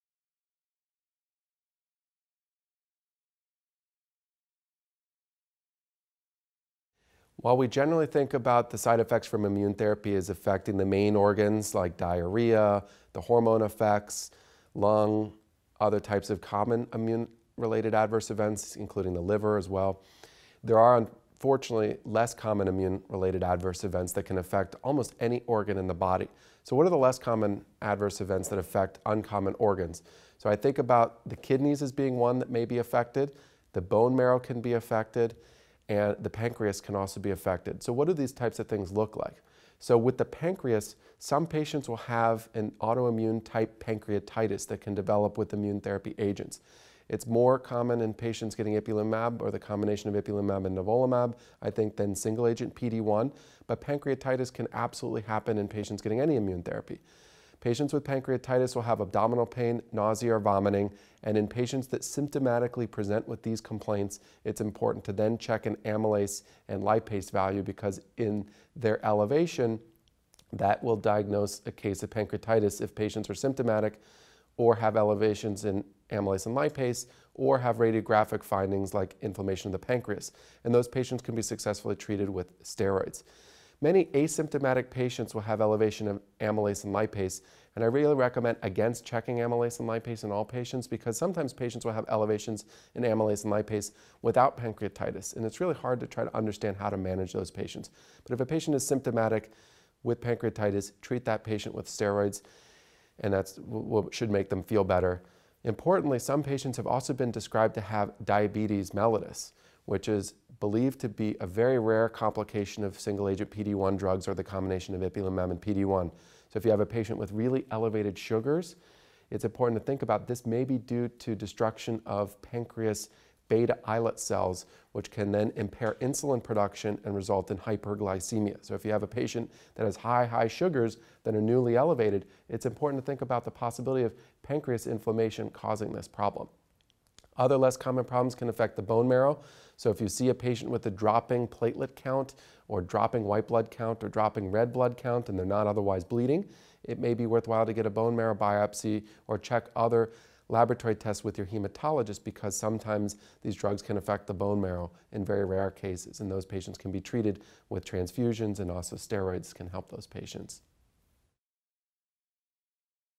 Less Common Side Effects With Immunotherapy in Melanoma
Michael A. Postow, MD, describes the less common immunotherapy-related toxicities, such as renal, pancreatic, neurologic, and hematologic side effects, that can occur in patients with melanoma.